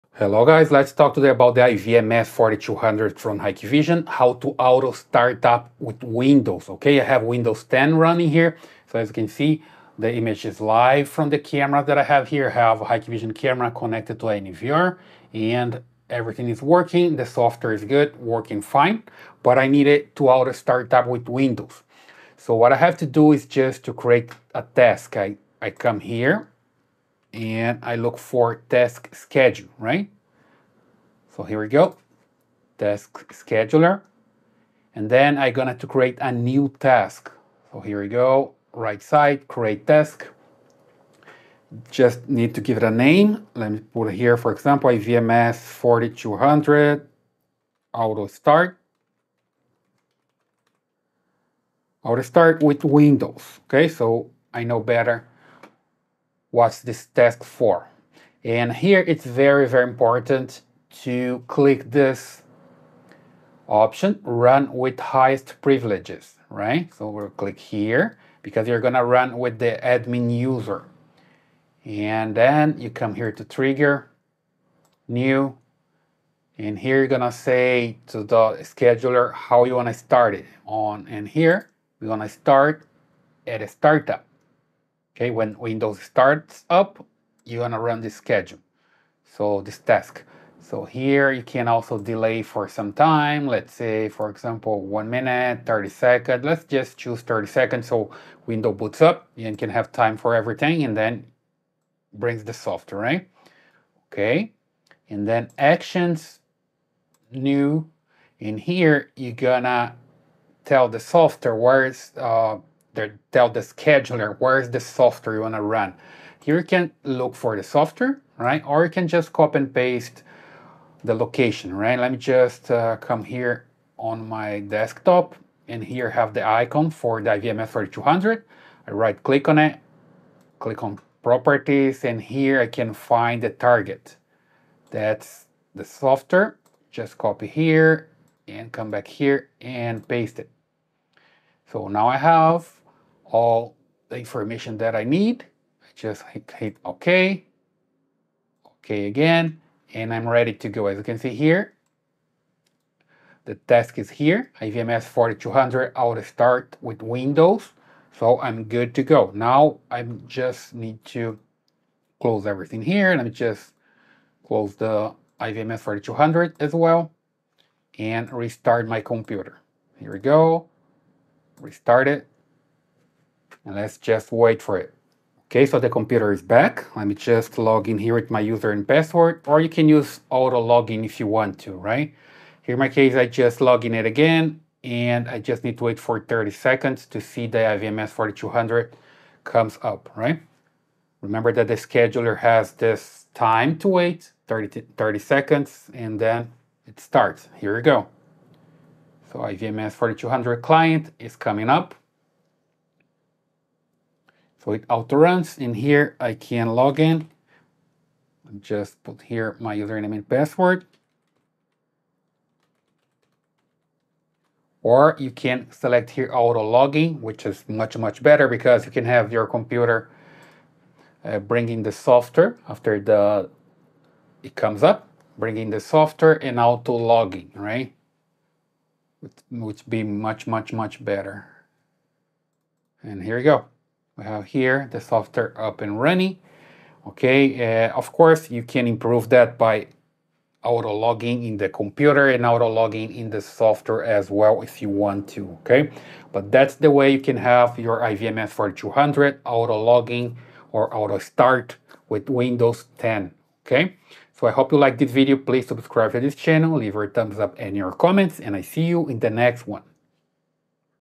iVMS 4200 Auto Start Up (Windows 10)
In this video, I show how to auto start the iVMS-4200 on Windows 10.
It's possible to use the Windows scheduler to have it working.
Watch this video to learn more about